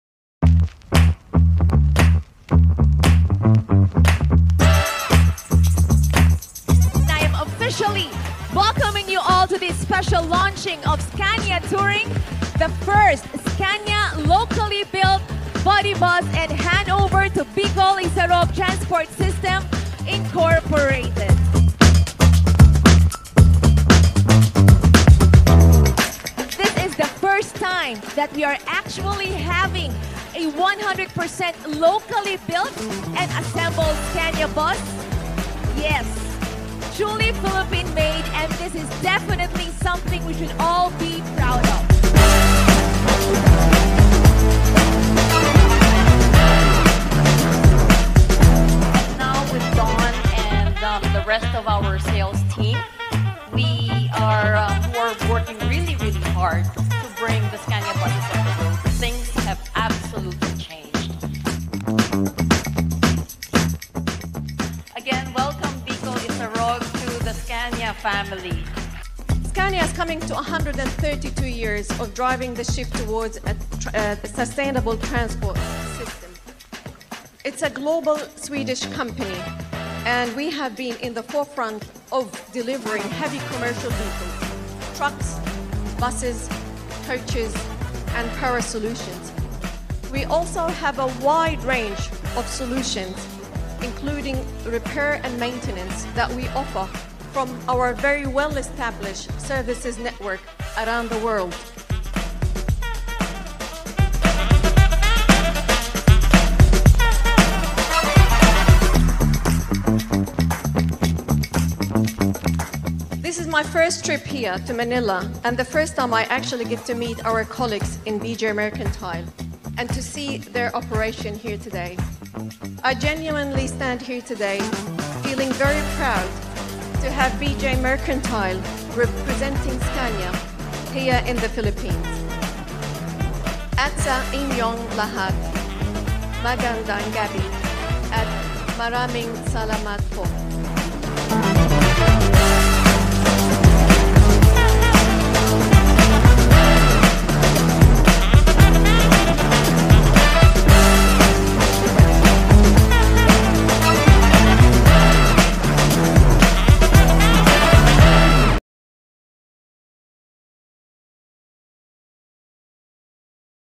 Scania unveils first locally built body to complement its new line of coach buses
Scania is unveiling the first Scania Touring with a Philippine-made body to be handed over to Bicol Isarog Tranport System, Inc.